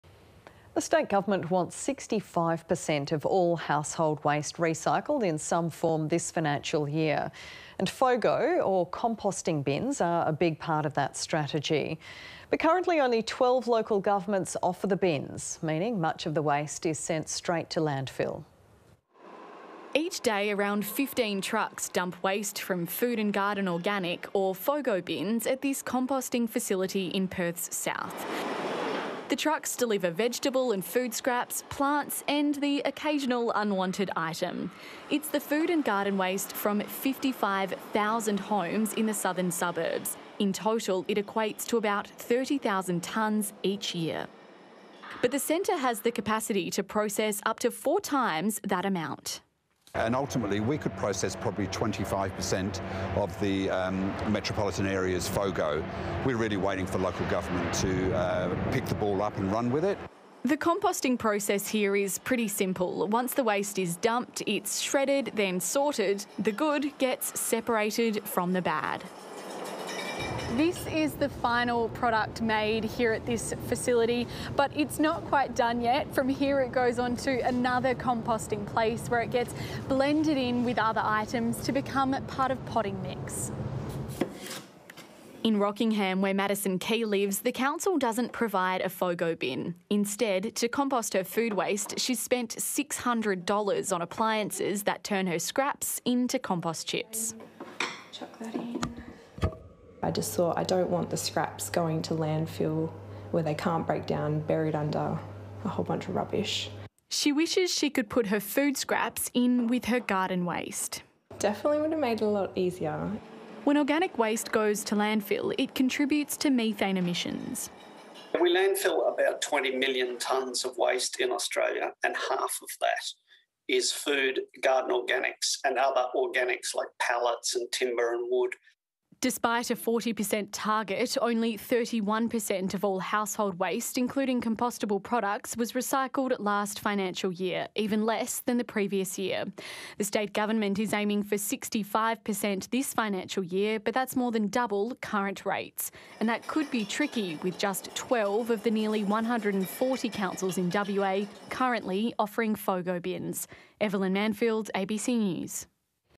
Push For Food Organics Garden Organics (FOGO) Composting Bins In WA Councils
(Source: ABC Australia, WA - 20 Sep 2021)